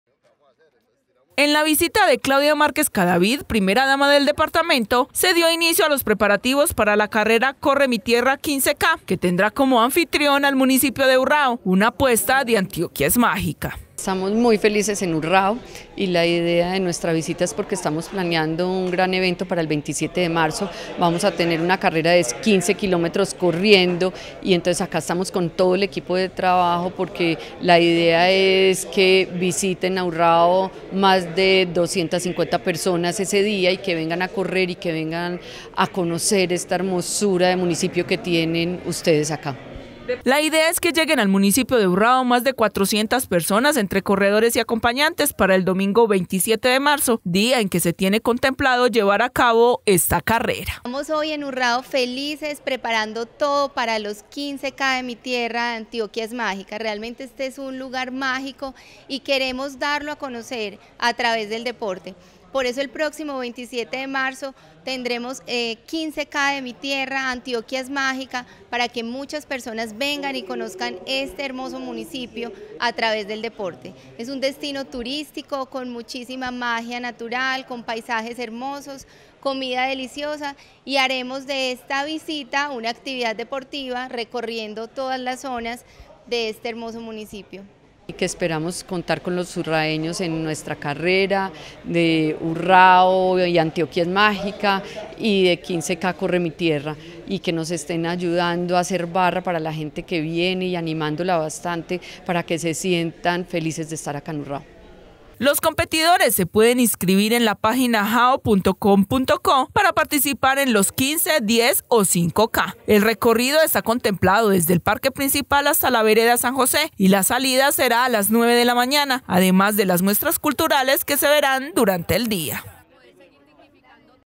CorreMiTierra 15K Urrao, de Antioquia es Mágica
La primera dama del departamento Claudia Márquez Cadavid estuvo en Urrao, con el fin de conocer el recorrido que se hará en la carrera que tendrá lugar el próximo 27 de Marzo en la localidad, gracias a una apuesta de Antioquia es mágica y Corre Mi Tierra 15k, para dar a conocer los territorios.